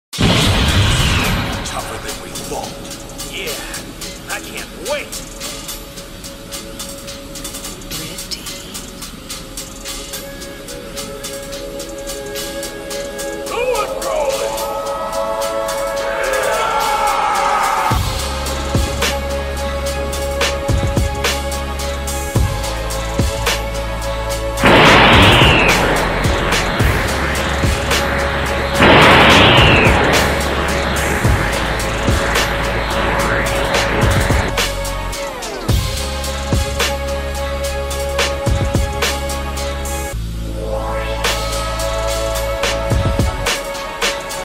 Goku and Vegeta Vs Broly Teaser
This is a short teaser for an animation that will be created once the sprites are ade, it will most likly be LSWI sprites tho
HUGE thanks dragonballspritefight for the vegeta sprite Check him out